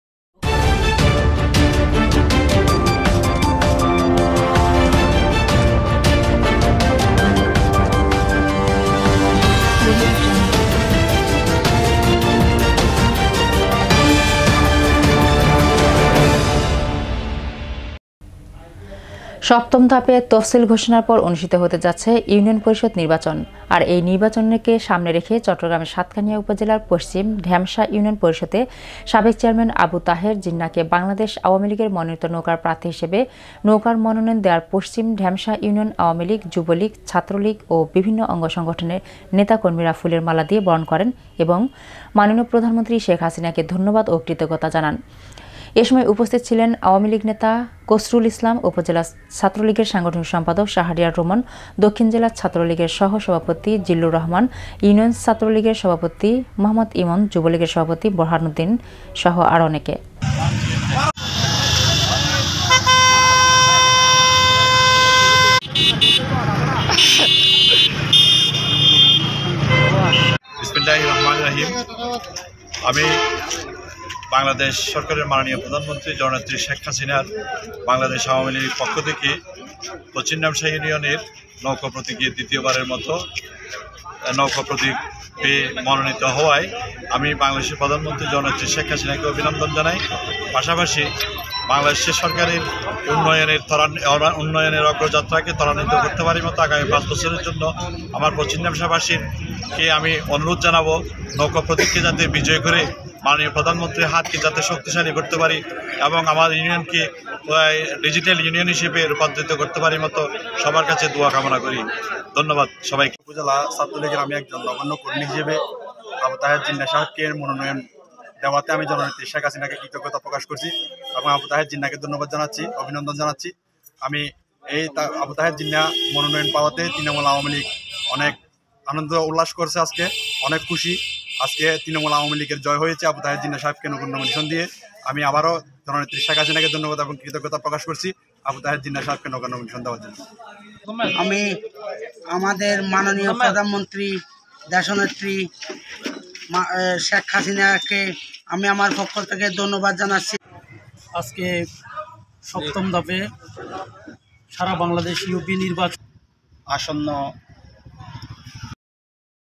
চট্টগ্রাম পশ্চিম ঢেমশার সাবেক চেয়ারম্যান জিন্নাহকে ফুলের মালা দিয়ে বরণ করেন নেতা কর্মীরাa2znewslive
সপ্তম ধাপে  তাফসীর ঘোষণার পর অনুষ্ঠিত হতে যাচ্ছে  ইউনিয়ন পরিষদ নির্বাচন এই নির্বাচনকে সামনে রেখে   চট্টগ্রাম সাতকানিয়া উপজেলার পশ্চিম  ঢেমশা ইউনিয়ন পরিষদের  সাবেক চেয়ারম্যান আবু তাহের জিন্নাহকে বাংলাদেশ আওয়ামী লীগের মনোনীত নৌকার প্রার্থী হিসাবে নৌকার মনোনয়ন দেওয়ার পশ্চিম ঢেমশা ইউনিয়ন আওয়ামী লীগ,যুবলীগ, ছাত্রলীগ ও বিভিন্ন অঙ্গ সংগঠনের নেতা কর্মীরা ফুলের মালা দিয়ে বরণ করেন এবং মাননীয় প্রধানমন্ত্রী শেখ হাসিনাকে ধন্যবাদ ও কৃতজ্ঞতা জানাই।